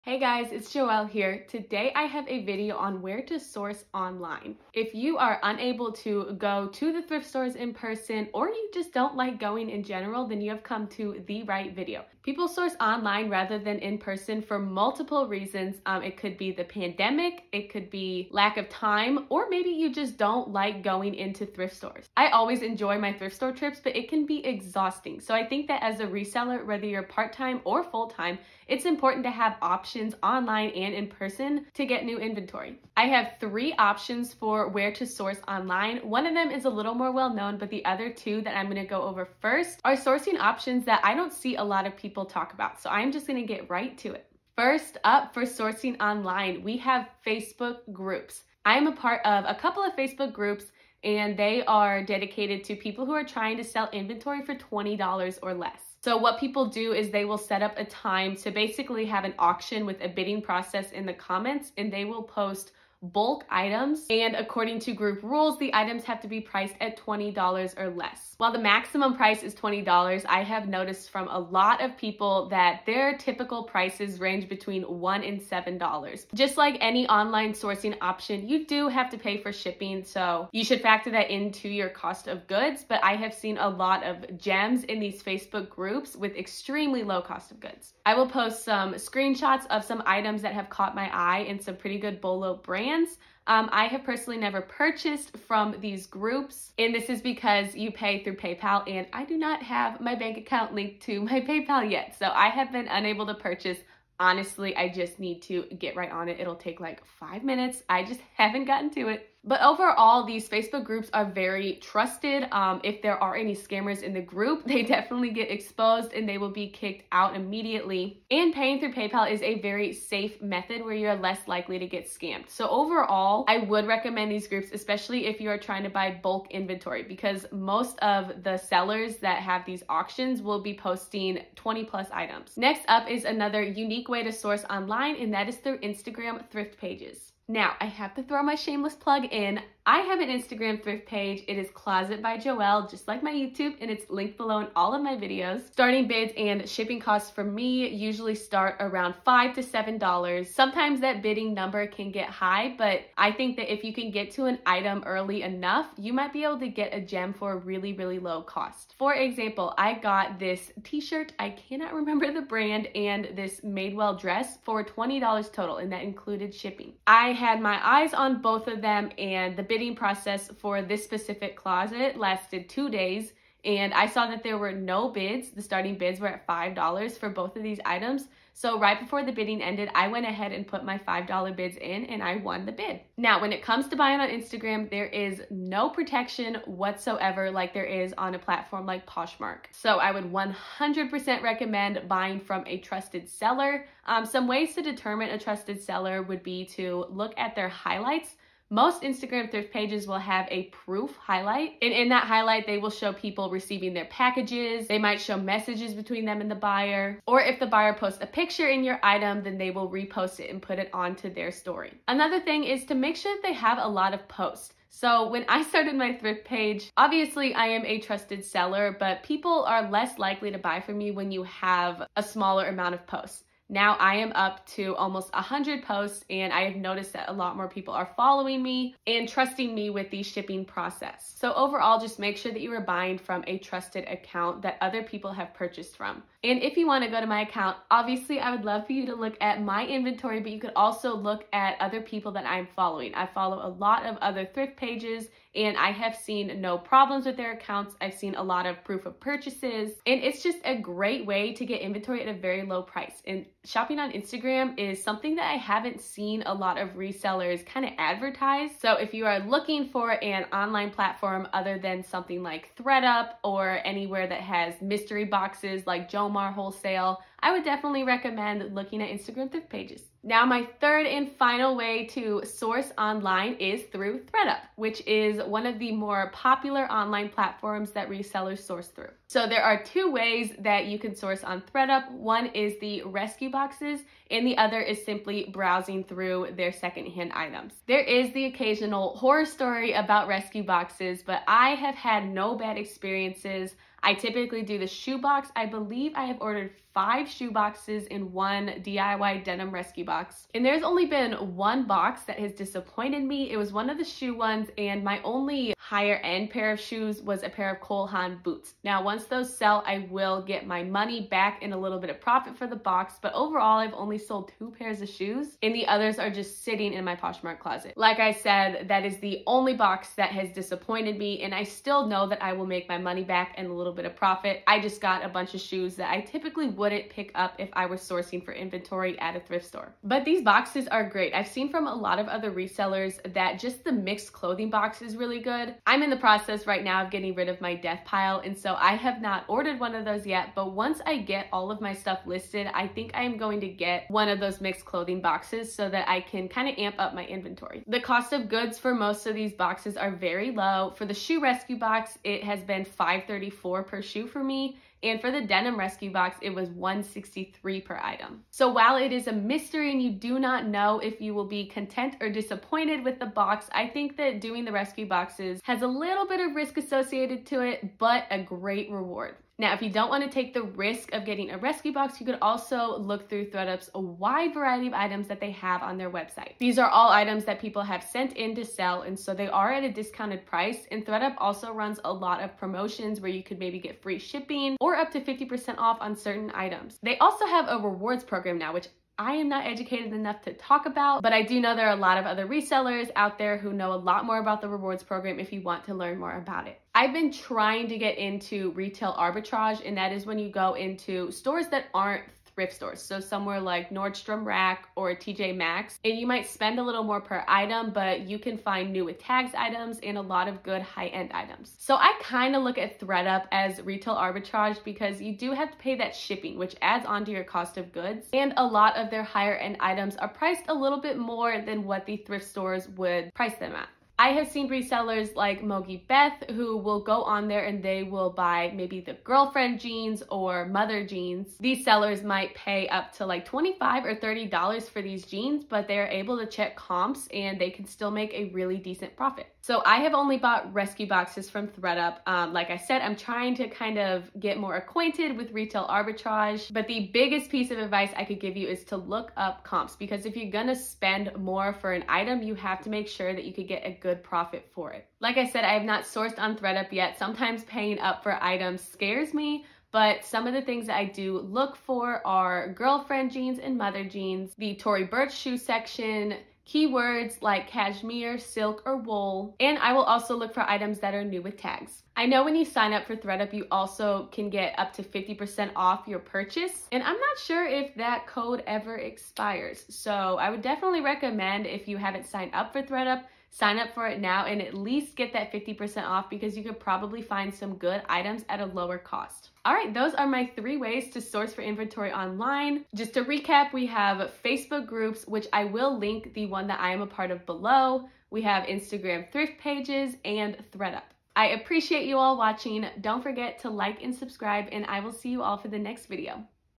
Don’t Have Time To Go To The Thrift Stores? Where To Source Online!
If you don’t have time to go to the thrift store, how can you get new inventory to resell on Poshmark or Mercari? In this video I go over various websites and platforms that allow you to source inventory online and have it shipped to your home. Furthermore, we are in the middle of a pandemic. When I tested positive for Covid, I needed to quarantine. While I was able to list and catch up on inventory management, I was unable to go thrifting and find new inventory. So I decided to find places to source inventory online (like ThredUp)!

But ThredUp is not the only option. In this video I will share with you several different ways to source online, while factoring in pricing, shipping and condition of items!

Instagram Thrift Store - Thrifted items for sale available now!

Sign up for Mercari and get up to $30.